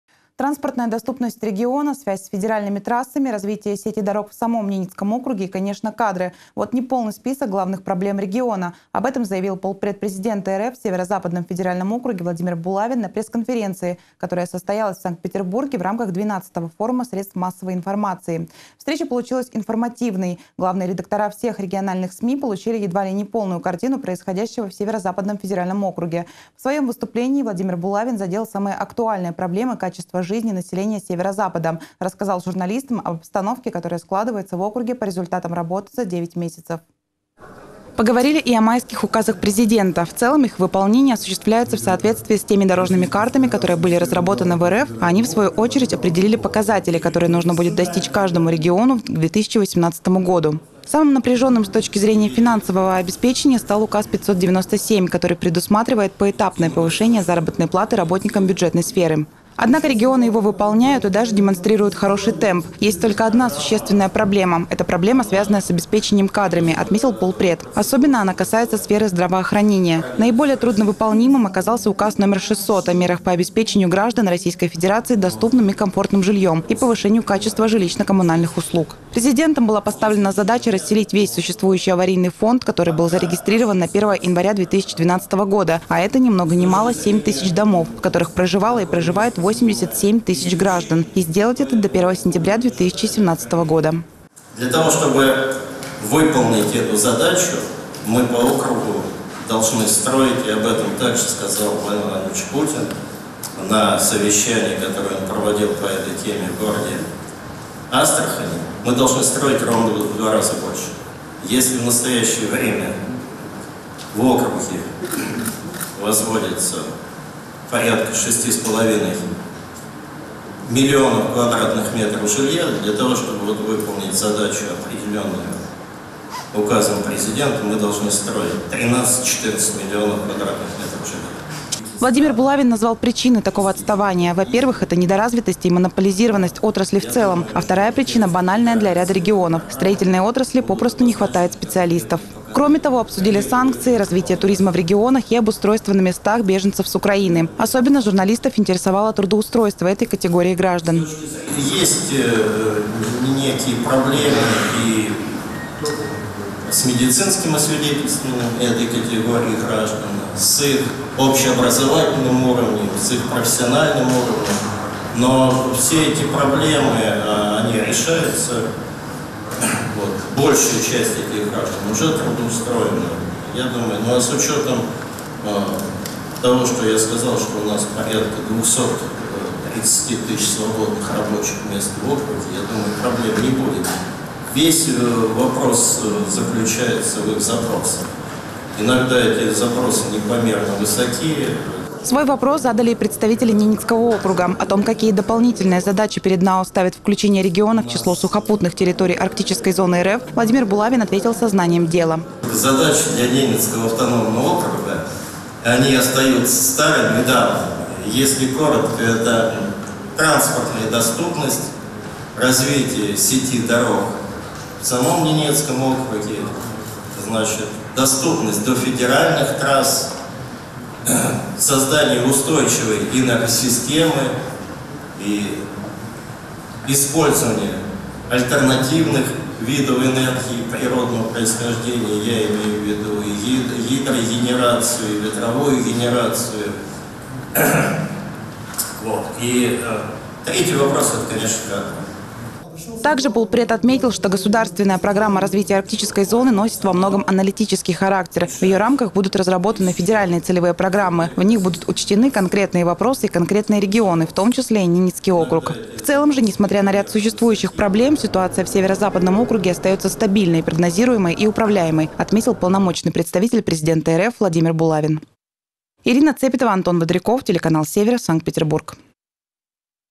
Владимир Булавин перечислил приоритетные направления в развитии НАО
Транспортная доступность региона — связь с федеральными трассами и развитие внутренней дорожной сети, освоение альтернативной энергетики и, конечно, кадры — вот те приоритетные задачи, которые стоят сейчас перед Ненецким округом, отметил  полпред президента РФ в СЗФО Владимир Булавин на пресс-конференции, прошедшей в Санкт-Петербурге в рамках XII Форума средств массовой информации.